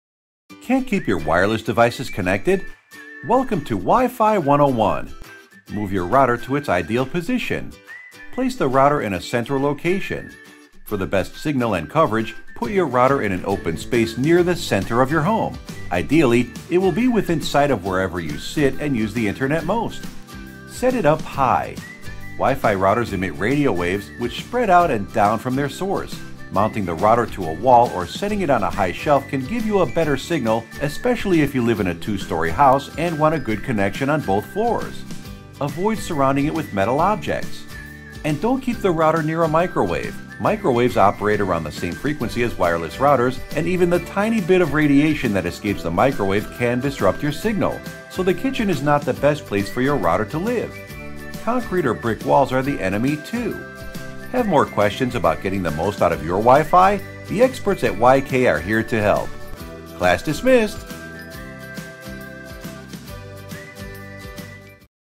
WiFi 101: Router Placement
This short video covers the topic of proper router placement within the home or office and problems that you can experience when you router is not positioned correctly or cannot provide the proper coverage for a larger home or office.